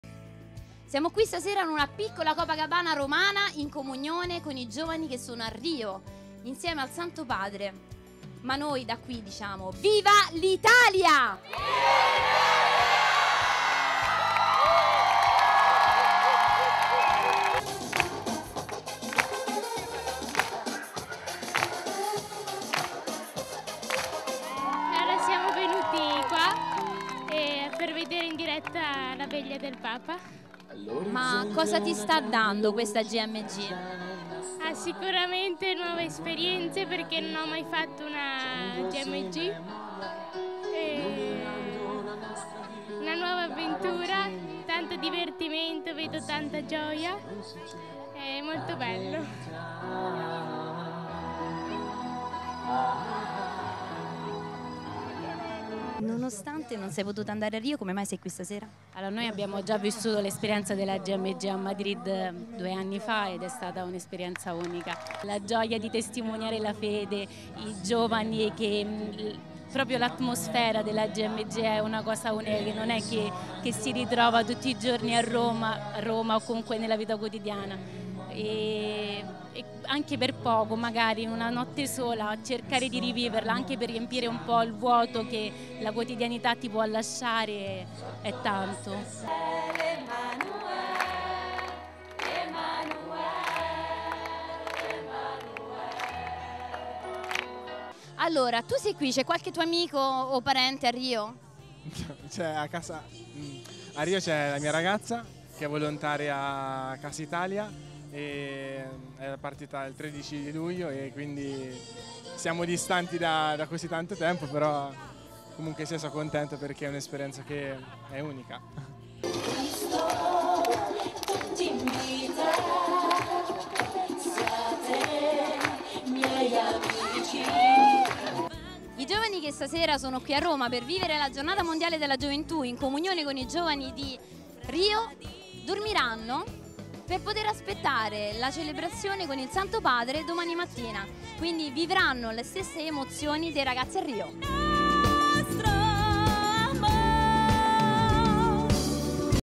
Veglia della GMG Rio 2013 a Roma
I ragazzi italiani che non sono potuti andare in Brasile per la GMG (Giornata Mondeale della Gioventù), la seguirono da Roma con la gioventù di tutto il mondo che era li a Rio de Janeiro.